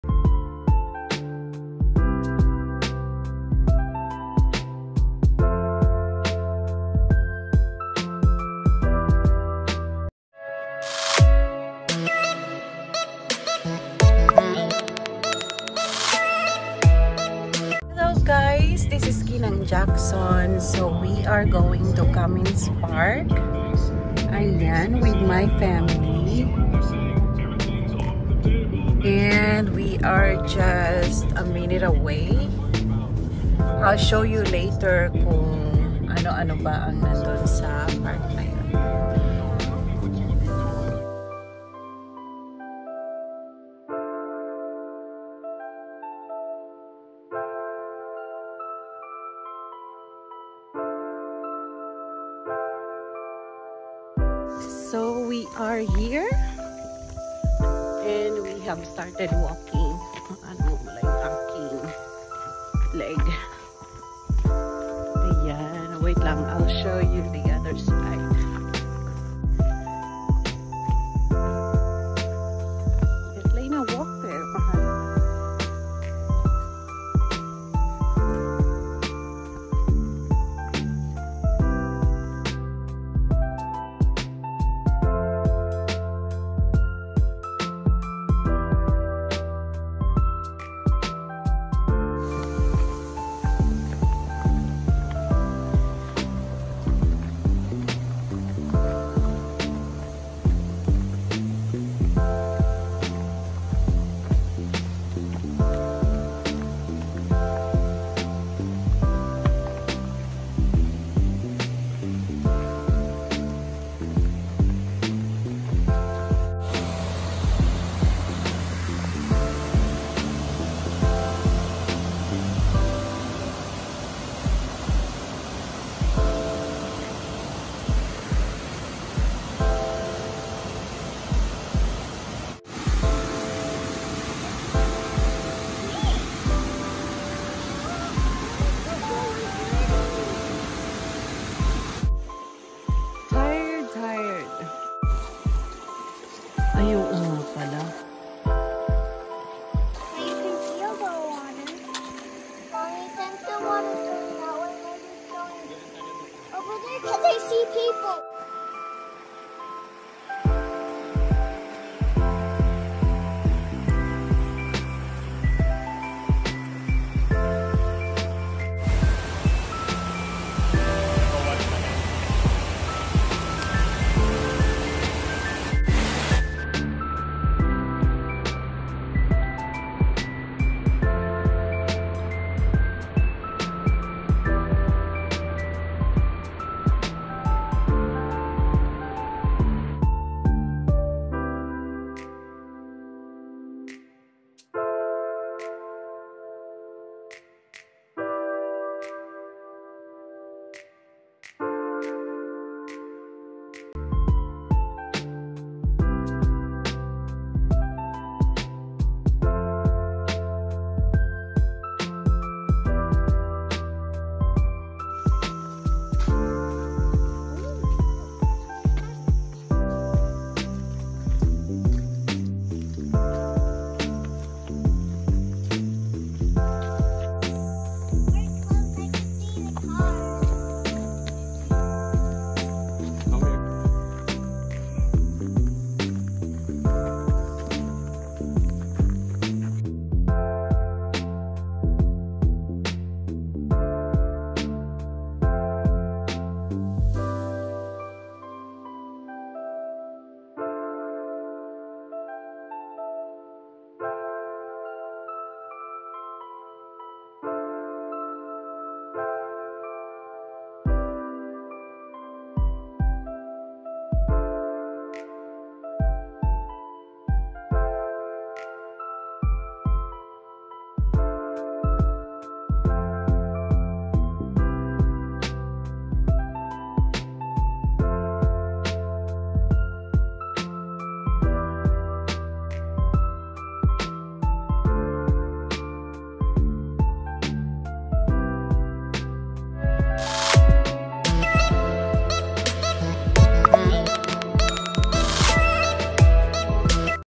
Trip to CUMMINS FALLS State Park | A very lovely waterfall | Hiking , long trail exercise
I'm so glad that my family had a chance to go on a trip to Cummins Falls State Park. It is a very lovely waterfalls and it has a long trail that can exercise you before you reach the main view of the waterfalls. Good way of hiking. Such a great and a nice recommendation to all the nature lovers to go to this wonderful place. My family enjoyed it so much and I am sure you guys can enjoy it too if you will have a chance to take a trip there. Visiting the Cummins Falls State Park here in Tennessee is  a great way to bond with your family as well.
 - Gng. Jackson